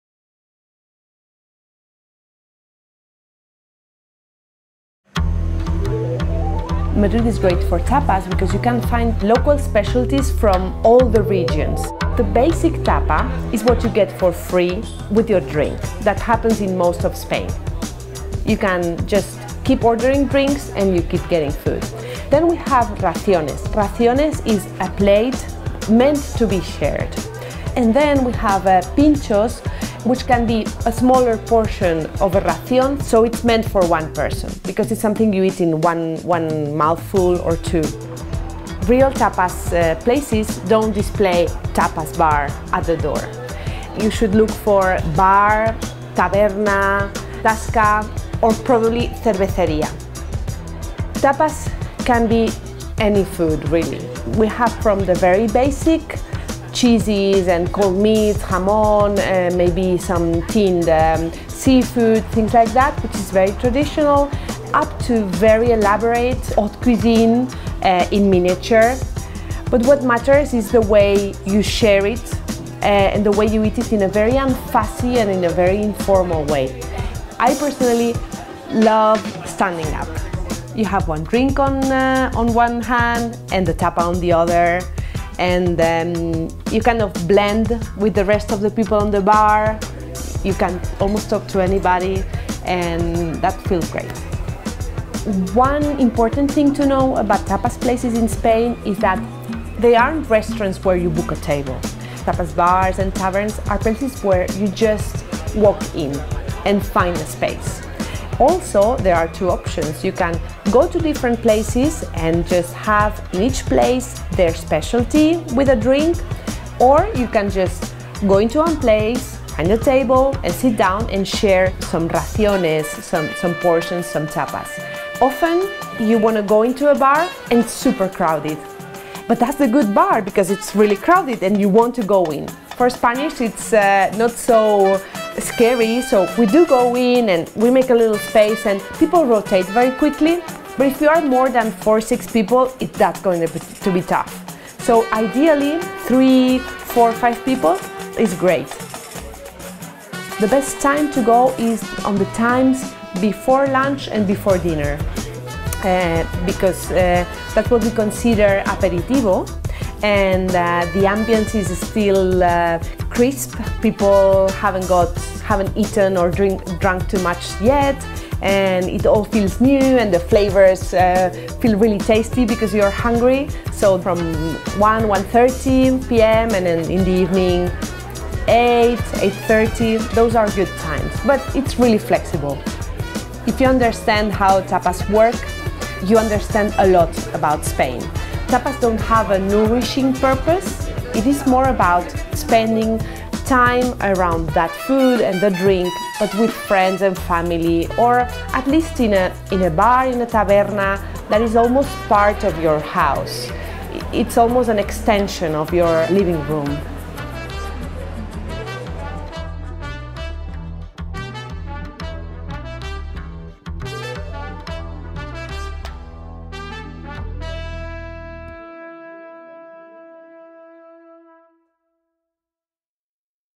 The secret to ordering tapas
We jaunt around Madrid’s tabernas, the lively living rooms of the Spanish capital, and share some tasty dishes with the regulars.